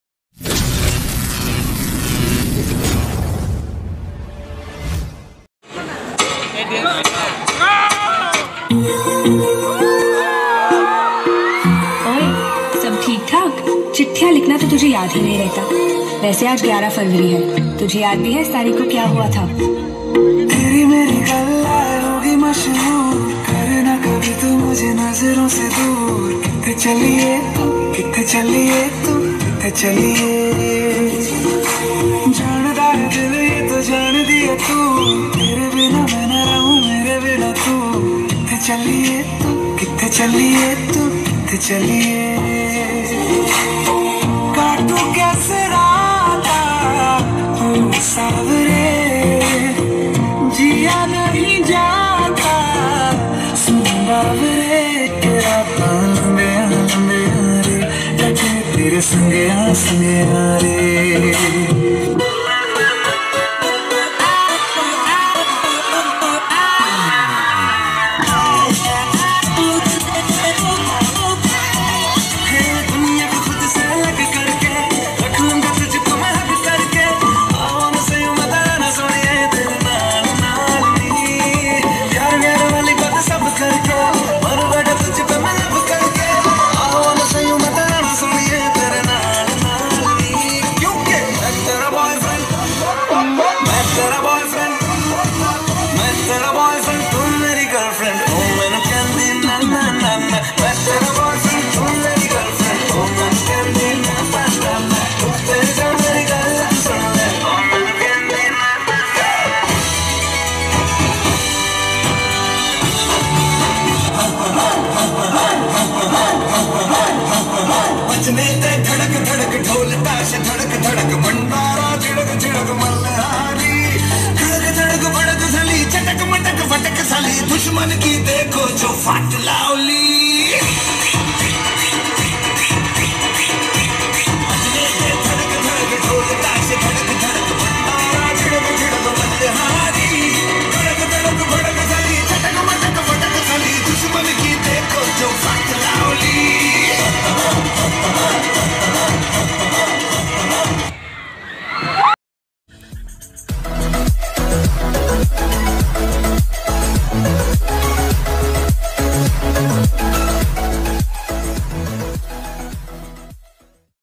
New Dance, Hindi song || Arjun Debnath
NEW DENCE , ARJUN DEBNATH . SAKTI ASHRAM H . S. AND VOC SCHOOL . FRESHMEN SOCIAL DAY . 10 SEPTEMBER 2022